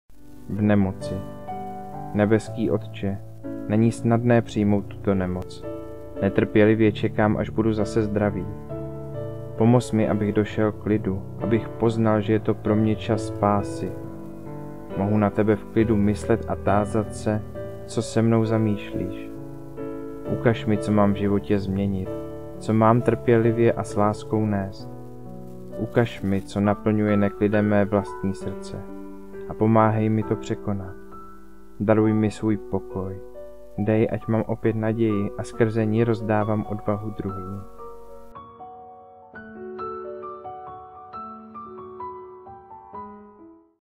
V nemoci
Nebeský Otče, není snadné přijmout tuto nemoc.
Netrpělivě čekám, až budu zase zdravý.
Pomoz mi, abych došel klidu, abych poznal, že je to pro mě čas spásy.
Mohu na tebe v klidu myslet a tázat se, co se mnou zamýšlíš.
Ukaž mi, co mám v životě změnit, co mám trpělivě a s láskou nést.
Ukaž mi, co naplňuje neklidem mé vlastní srdce, a pomáhej mi to překonat. Daruj mi svůj pokoj.
Dej, ať mám opět naději a skrze ni rozdávám odvahu druhým.